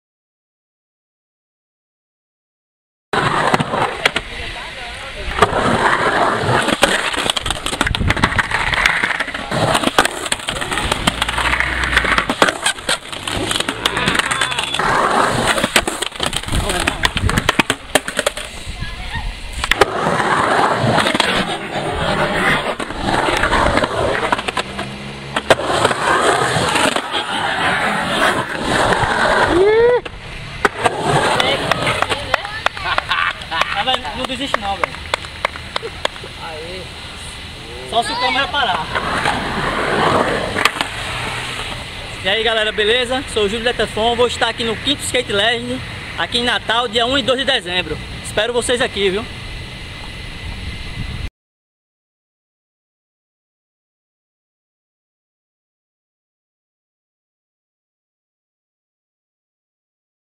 CHAMADA V NORDESTE SKATE LEGENDS.JÚLIO DETEFON
O Skatista Sergipano Júlio Detefon convida todos para estarem nos dias 1 e 2 de Dezembro de 2012 no V Nordeste Skate Legends em Parnamirim, região metropolitana de Natal-RN. A competição do evento contará pontos para o ranking VeteranSkater 2012

Imagens e Edição: Paulo Costa
Arte: Zé Mário